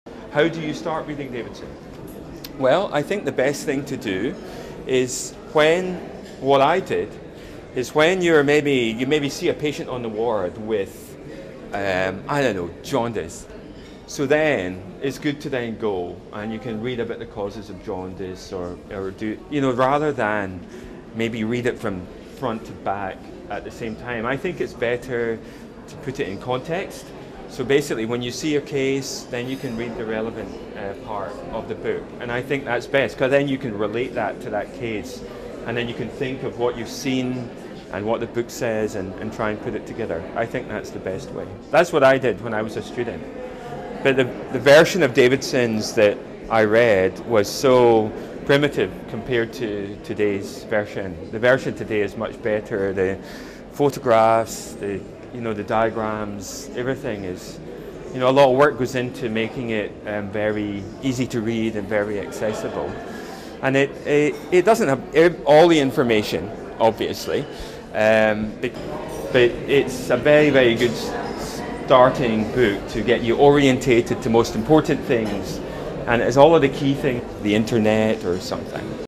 How do you start reading Davidsons.mp4
Stuart H Ralston giving some tips to Students on How to start reading Davidsons